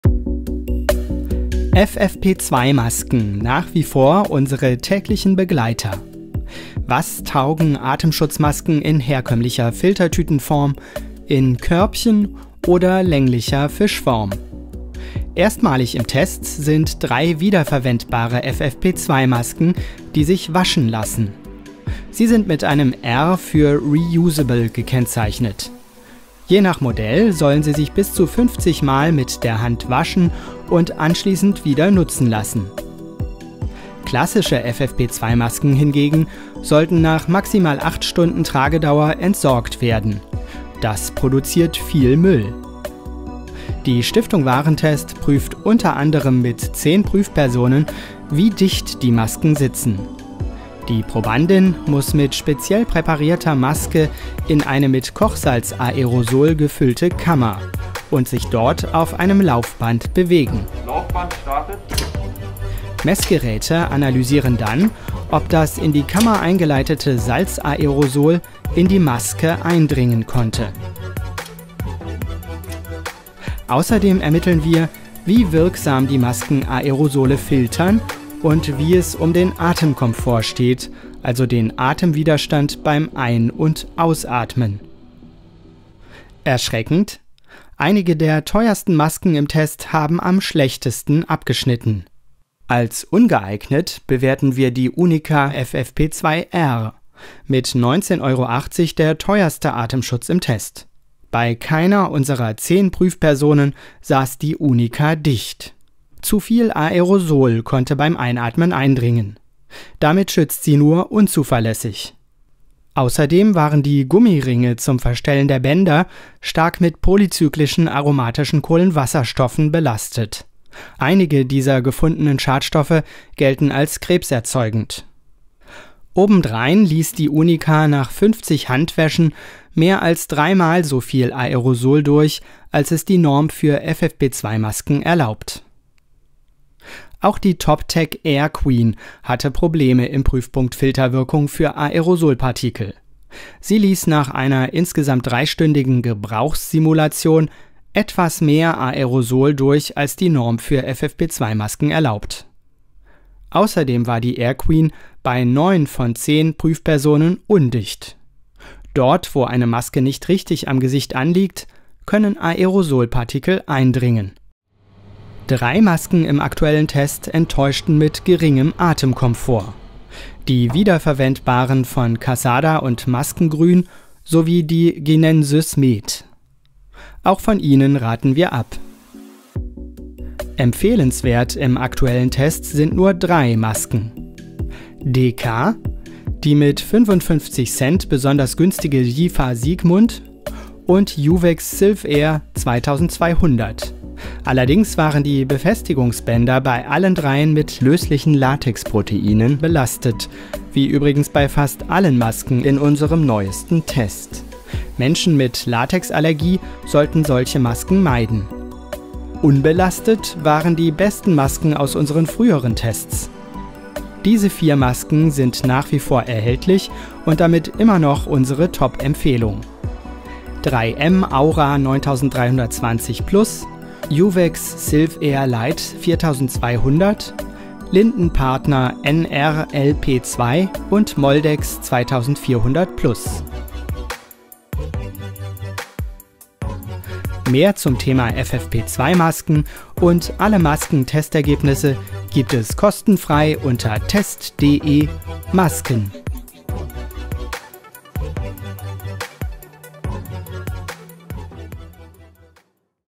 Test FFP2-Masken 2022: Die besten Atemschutzmasken & welche Ihr meiden solltet - Stiftung Warentest
FFP2-Masken im Test: Die Filterwirkung ist meist gut, manche können das Atmen aber stark beein­trächtigen oder sitzen nicht bei jedem optimal. Waschbare FFP2 R Masken enttäuschten.
00:00 - FFP2-Masken im Test
00:15 - Waschbare, wiederverwendbare FFP2-Masken erstmals im Test
00:43 - Wie dicht sitzen die FFP2-Masken?
01:12 - Test der Filterwirkung und des Atemwiderstands von FFP2-Masken
01:32 - Teuerster Atemschutz ist am schlechtesten: Unica FFP2 R im Test
02:16 - Test Toptec Air Queen - schlechte Filterwirkung & oft undicht
02:45 - Geringer Atemkomfort bei 3 Masken
03:00 - FFP2-Masken Testsieger im aktuellen Test
03:13 - Diese FFP2-Masken bei Latex-Allergie vermeiden
03:29 - Die 4 besten FFP2-Masken im Test
Neue Masken Test 11/2022:
• DK FFP2 NR D YSK-P2D
• Jifa Siegmund FFP2 NR Atemschutzmaske JFM02
• Uvex FFP2 NR D silv-Air 2200
• Hard FFP2 Atemschutzmaske Spirit One 77017
• Lindenpartner FFP2 partikelfiltrierende Halbmaske NR LP1
• Opharm FFP2 Schutzmaske
• Thorey Atemschutzmasken FFP2 NR Falt Basic Plus
• Ginensys med FFP2 NR GM-03
• Toptec Air Queen Breeze Mask FFP2 NR
• Casada FFP2 R Nonomask Pro
• Maskengrün Atemschutzmaske FFP2 R D
• Unica FFP2 R
Die Stiftung Warentest hat aktuell ihren dritten FFP2-Masken-Test abge­schlossen. In unseren ersten beiden Unter­suchungen, die im Februar und Juli 2021 auf test.de erschienen, konnten wir nur vier von zwanzig Masken ohne Einschränkung empfehlen. Dieses Jahr schickten wir zwölf weitere FFP2-Masken ins Labor. Mit dabei waren auch drei Modelle, die als wieder­verwend­bar gekenn­zeichnet sind. Die wichtigsten Ergeb­nisse unserer drei Tests von nunmehr 32 Masken im Über­blick:

Die empfehlens­werten Masken. Rundum mit „geeignet“ empfehlen können wir nach wie vor nur vier FFP2-Masken, die wir im Jahr 2021 geprüft und veröffent­licht haben: je ein Modell von 3M, Lindenpartner, Moldex und Uvex. Alle vier schützen sehr gut vor Aerosolen, bieten genug Atem­komfort, über­zeugen in Pass­form und Dichtig­keit und schneiden unauffäl­lig in der Schad­stoff­prüfung ab. Die Masken sind laut Anbieter unver­ändert und zu den in unserer Test­tabelle genannten (regel­mäßig aktualisierten) Preisen erhältlich.

Das beste Preis-Leistungs-Verhältnis. Die Linden­partner FFP2 NR LP2 kostet nur 50 Cent pro Stück, die Uvex FFP2 NR silv-Air lite 4200 nur 67 Cent. Sie gehören zu den vier Besten. Zum Vergleich: Die 3M-Maske hat einen Stück­preis von 2,74 Euro.

Welche Masken auch geeignet sind. Noch fünf weitere FFP2-Masken aus allen bisherigen Tests können wir zwar empfehlen. Aber wegen sehr geringer Allergierisiken durch lösliche Latexpro­teine in den Bändern beur­teilten wir sie als „auch geeignet“, unsere zweit­beste Bewertungs­stufe. Allergiker sollten die Modelle von DK, Dräger, Hum, Jifa und Uvex vorsichts­halber nicht tragen.

Wieder­verwend­bare FFP2-Masken nicht empfehlens­wert. Die Modelle von Casada, Maskengrün und Unica gehören zu einem Masken­typ, der mit „R“ gekenn­zeichnet ist. Das steht für „reusable“, also wieder­verwend­bar. Anders als klassische FFP2-Einmalmasken, die nach maximal acht Stunden Tragedauer entsorgt werden sollen, lassen sich R-Masken mit der Hand waschen und sollen danach wieder einsetz­bar sein. Letzt­lich über­zeugte keine der drei Mehrweg-FFP2-Masken im Test, sei es weil sie nur geringen Atem­komfort boten oder wie die Unica FFP2 R im Pass­form- und Schad­stoff­test durch­fielen. Als erste von uns geprüfte FFP2-Maske erhielt sie die Bewertung unge­eignet.

Tipp: Nur kurz getragene FFP2-Einmalmasken müssen Sie nicht unbe­dingt sofort entsorgen. Für den privaten Gebrauch können Sie solche FFP2-Masken aufbereiten − dabei gilt es, ein paar Dinge zu beachten.

Bei diesen Masken bekommen Sie schlecht Luft. Zehn Masken haben wir als wenig geeignet bewertet, weil bei ihnen der Atem­komfort gering war. Betroffen sind die Einmalmasken von Ginensys med, Hygisun, Kingfa, Leikang, Mea Vita, Taidakang, Mivolis von dm und Altapharma von Ross­mann. Gleiches gilt auch für die wasch­baren FFP2-Masken von Casada und Maskengrün. Wer sie trägt, bekommt schlechter Luft als unter Masken mit hohem Atem­komfort.

Wegen der Pass­form nur einge­schränkt geeignet. In den drei Tests haben wir nur 22 der 32 Masken auf Pass­form geprüft, weil die anderen schon vorher durch die Atem­komfort-Prüfung gefallen waren. 12 dieser 22 Masken erfüllen nicht alle Norm-Anforderungen in der Prüfung auf Pass­form und Dichtig­keit. Die Unica versagte in dieser Disziplin sogar voll­ständig. Sie saß bei keiner der zehn Prüf­personen dicht.